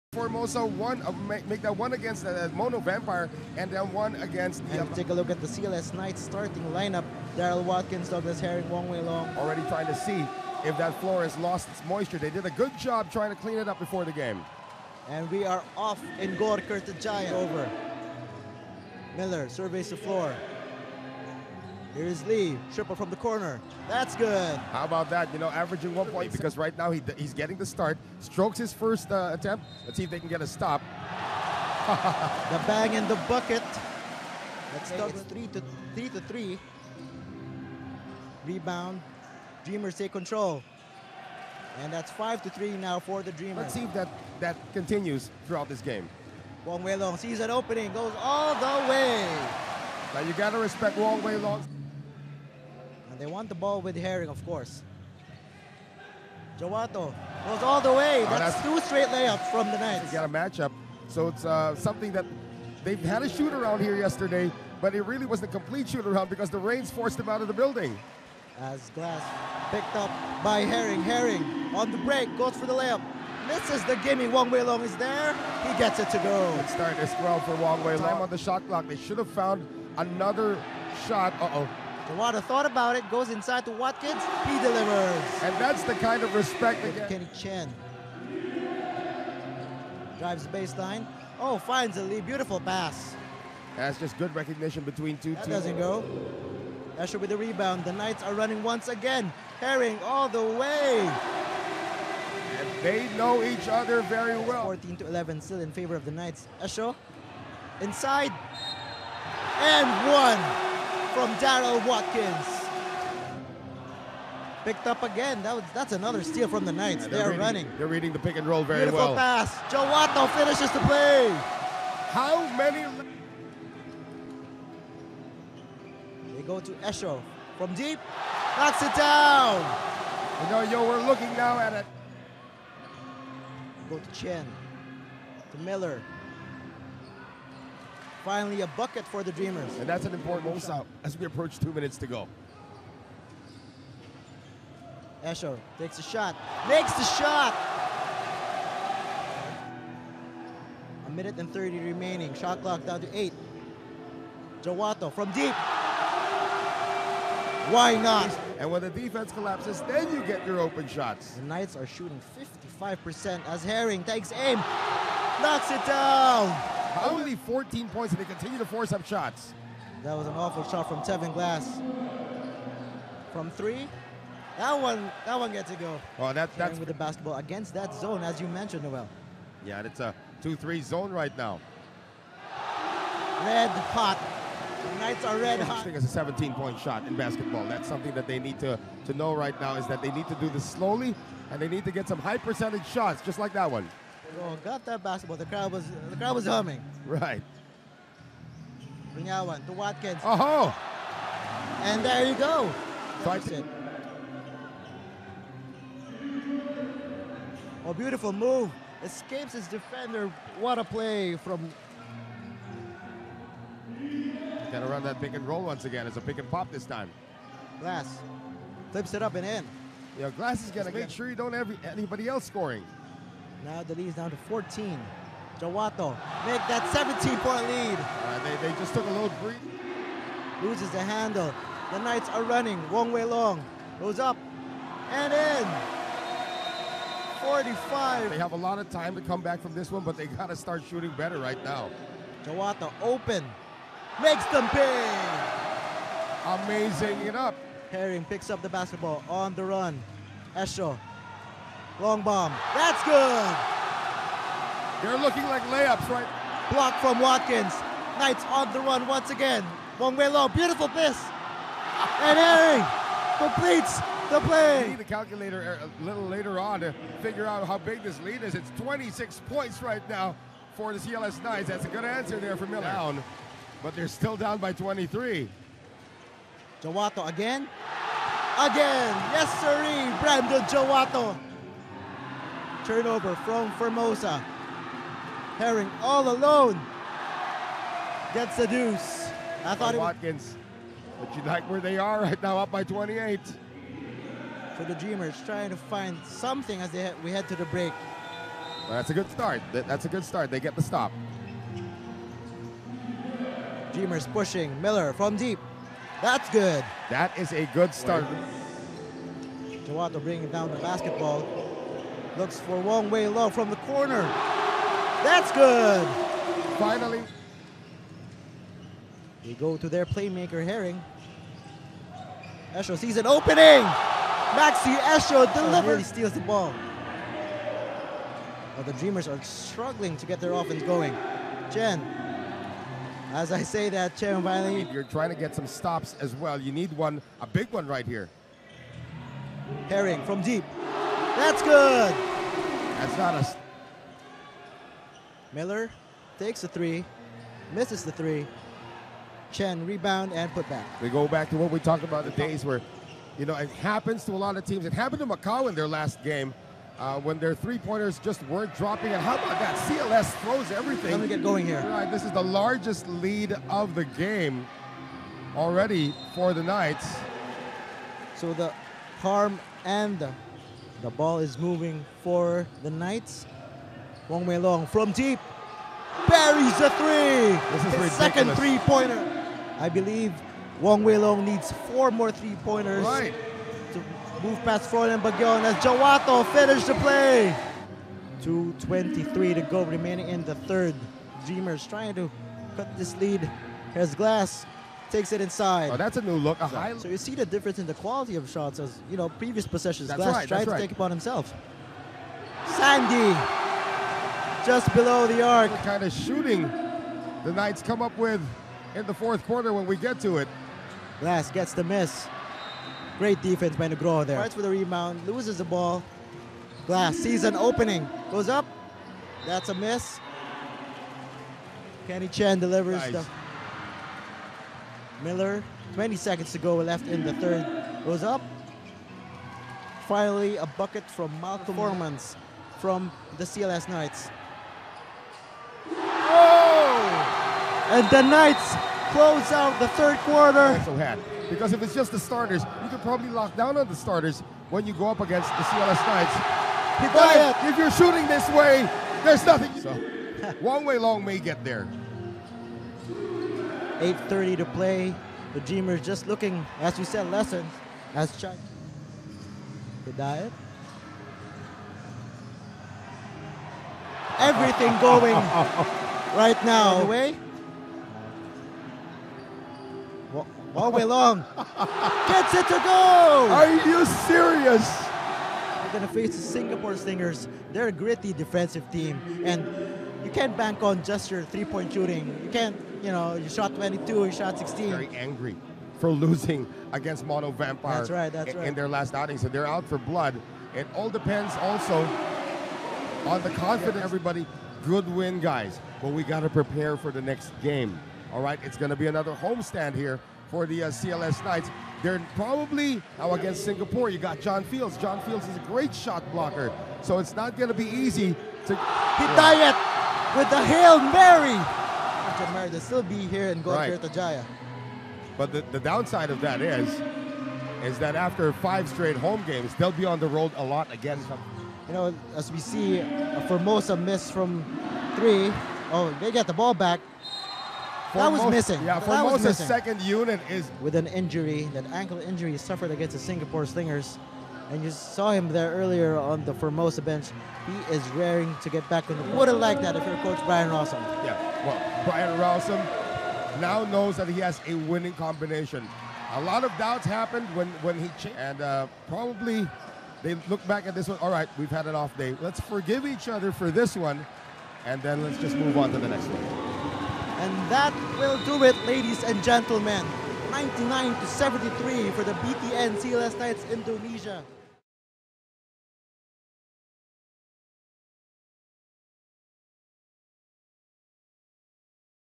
BTN CLS Knights vs Formosa Dreamers - Full Game Highlights | January 16, 2019 | ABL 2018/19
Bank BTN CLS Knights Indonesia (panjang banget ya namanya) berhasil balas dendam kepada Formosa Dreamers. Tim asuhan Brian Rowsom ini berhasil membantai Formosa dengan skor 99-73. CLS sendiri bermain amat baik pada hari ini, mereka bekerja sama sebagai tim, serta memiliki field goals percentage yang super bagus. Kerja bagus tidak hanya dilakukan oleh para pemain, coaching staff, dan para fans, para petugas pembersih lapangan / petugas ngelap lapangan juga bekerja sangat baik dalam game ini, bila dilihat, mereka sanagt sigap terjun ke lapangan bila terdapat basah di lapangan, kerja mereka yang sigap ini merupakan saah satu penyebab juga mengapa pertandingan bisa berjalan baik di hari ini :). Wong Wei Long dan Made Brandon Jawato menjadi pencetak points terbanyak bagi CLS, mereka berdua mencetak masing-masing 18 points, sedangkan di deretan pemain lokal, Arif Hidayat mencetak career highnya di ABL, yaitu 12 points. Dari kubu Formosa Dreamers, Malcolm Miller menjadi pengumpul poin terbanyak dengan 14 poin, 10 rebounds dan 3 asis.